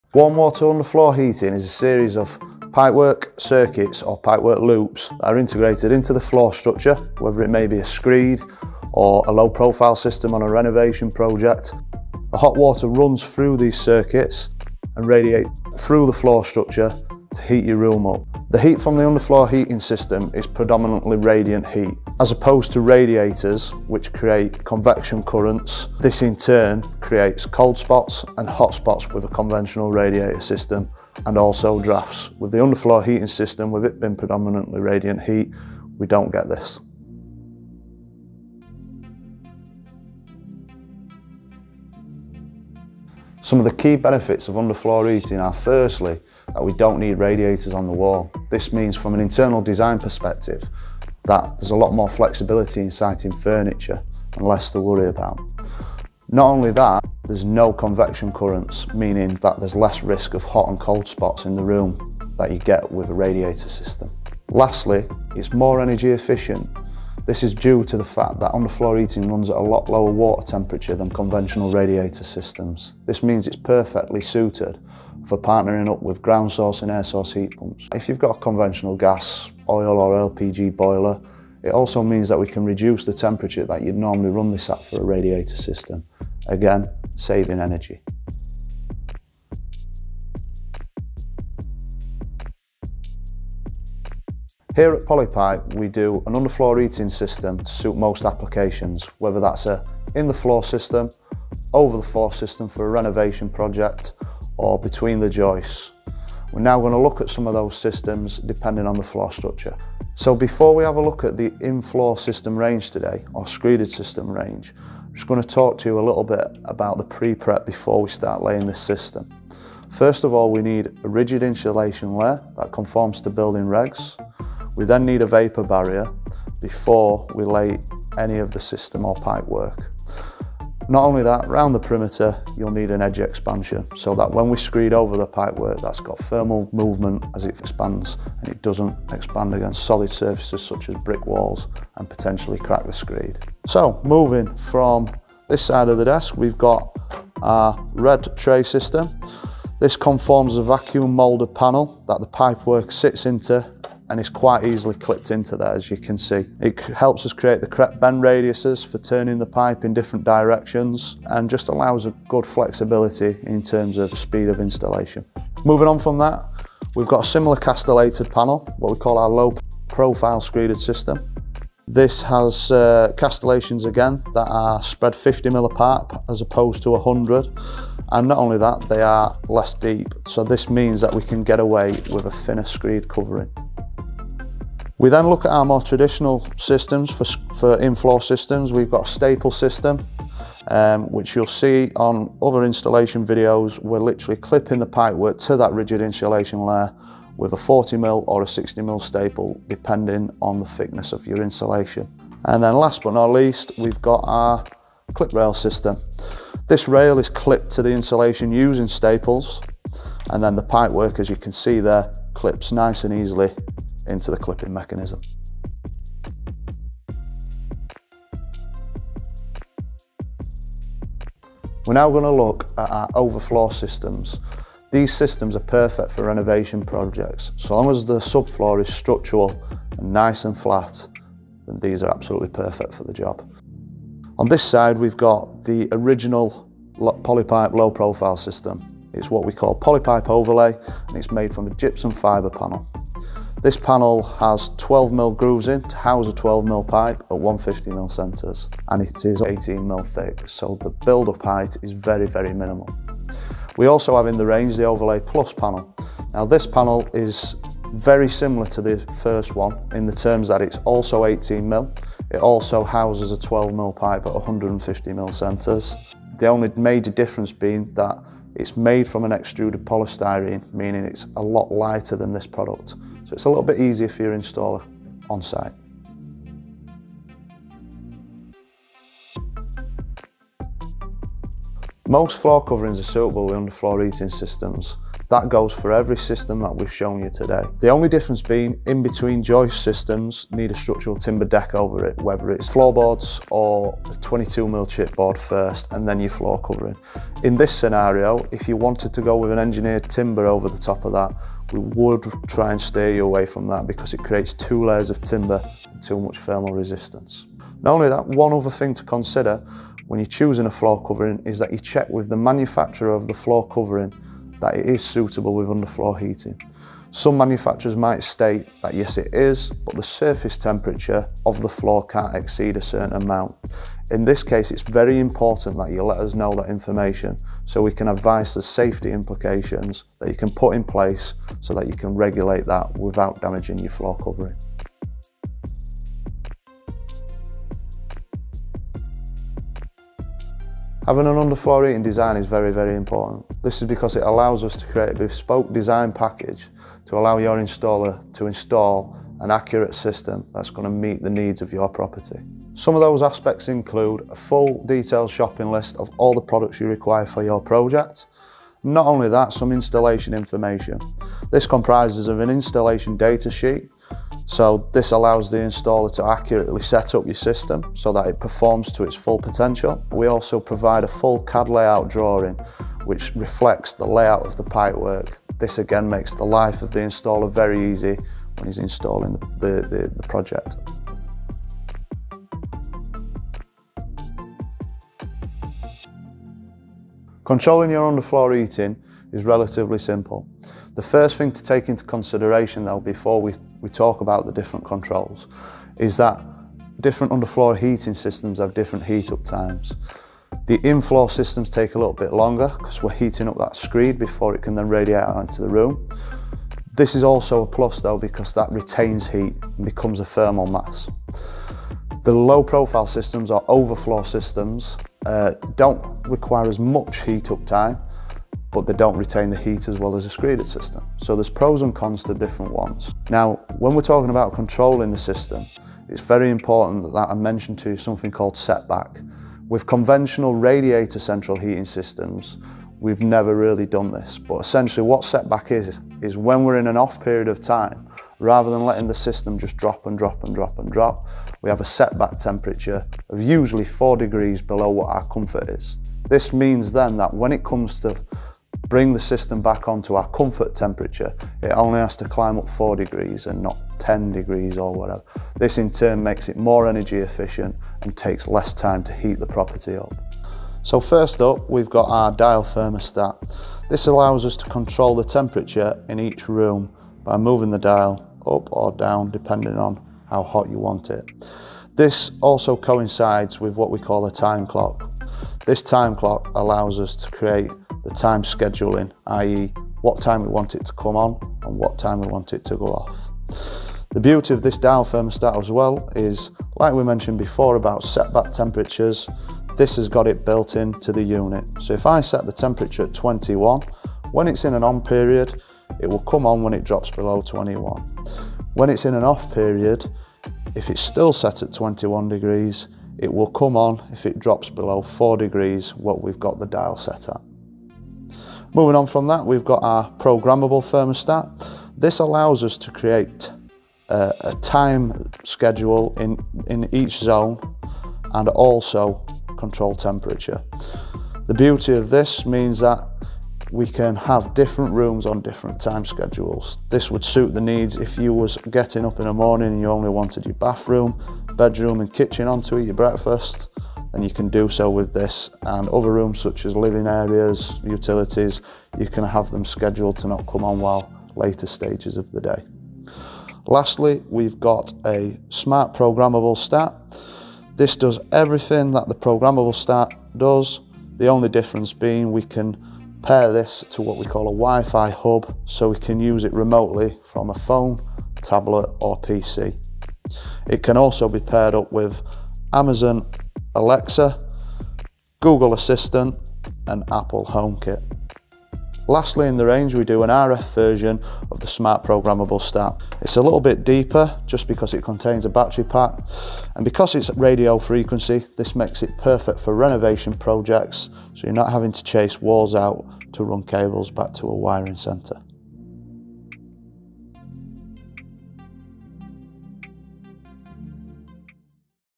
Warm Water Underfloor Heating explained! | Benefits, Floor Coverings, UFH Systems & Controls
Underfloor heating can do far more for you and your home than you think. It allows you more freedom over the design and temperature of your rooms than ever before.

This video has several sections, outlined as below:

0:00 What is warm water underfloor heating and how does it work?
0:48 The benefits of underfloor heating
1:39 The systems: In the floor
3:47 The systems: Over the floor
4:47 Advice on floor coverings
5:44 The importance of design
6:30 A walk through controls

Our underfloor heating experts are on-hand whatever your requirements.